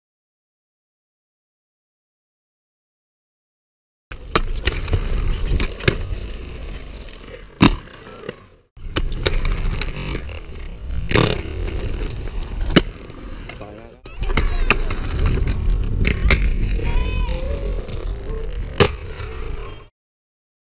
Dawid Kucharek Rolki Bielawa BRF
Jeden Dawid. Jeden murek . Trzy triki